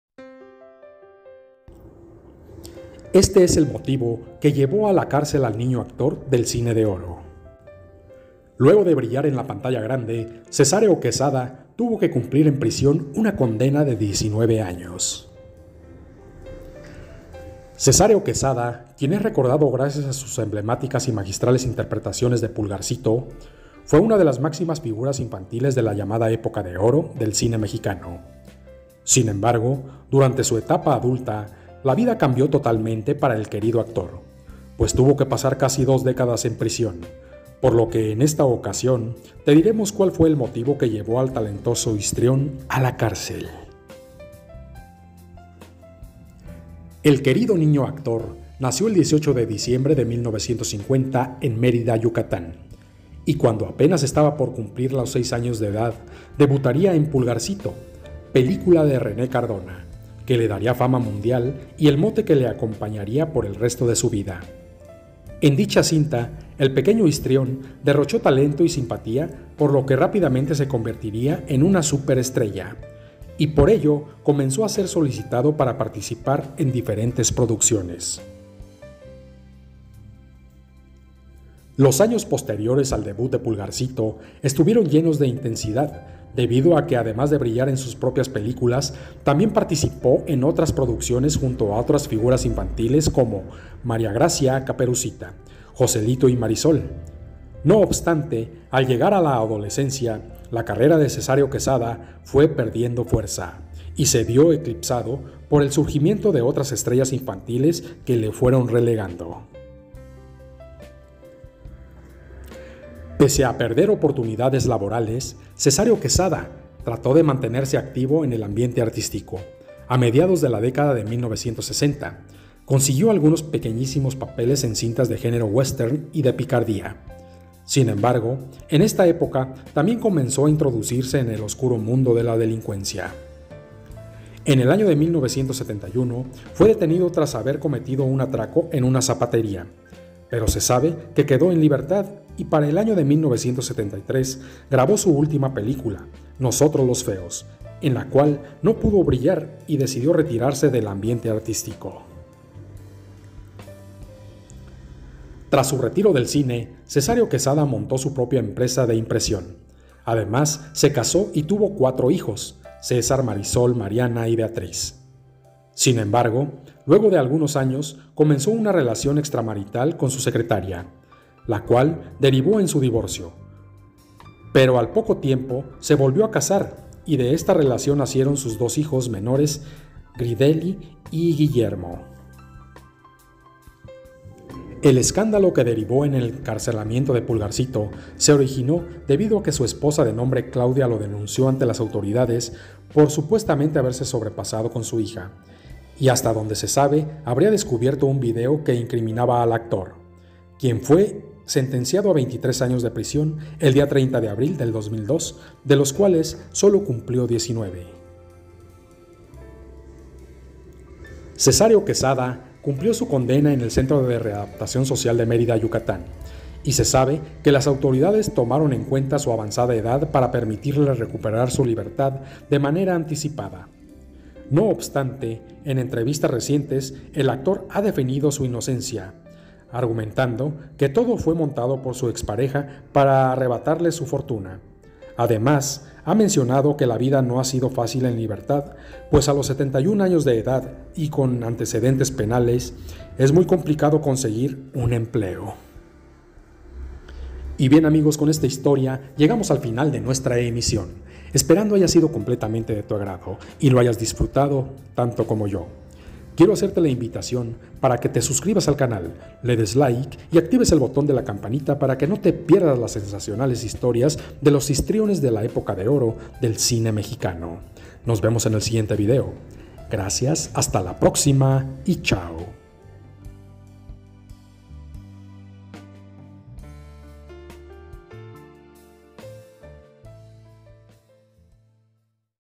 Los motivos que llevaron a PULGARCITO a Prlslón.
La tragedia que envolvió a niño Actor que dejo huella en el cine de oro mexicano con su papel de pulgarcito.